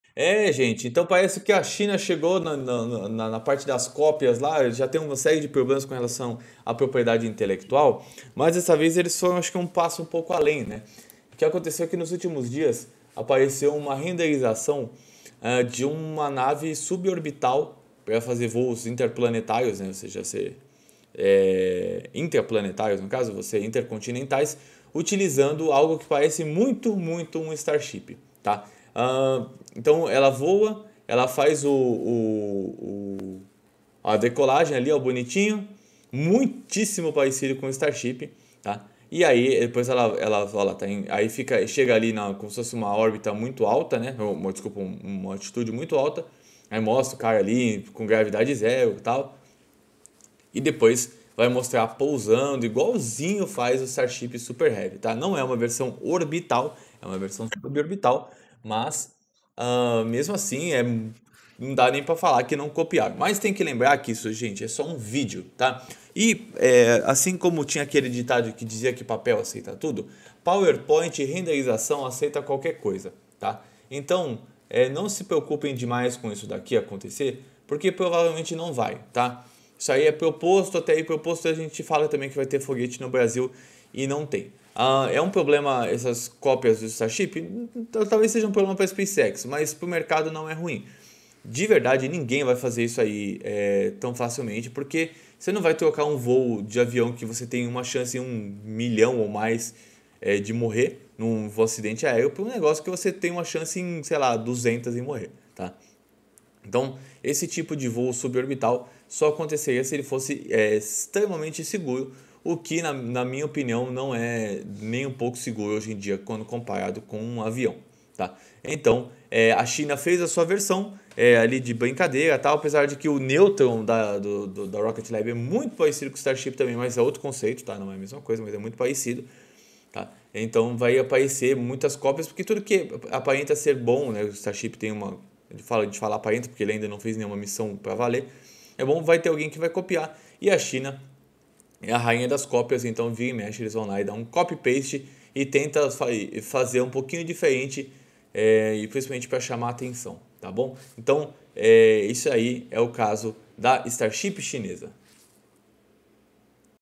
China Cópia Starship e Promete Voos Suborbitais Comerciais
Hoje falaremos sobre a Starship chinesa que surpreendeu o mundo e pretende fazer um veículo suborbital para transporte intercontinental.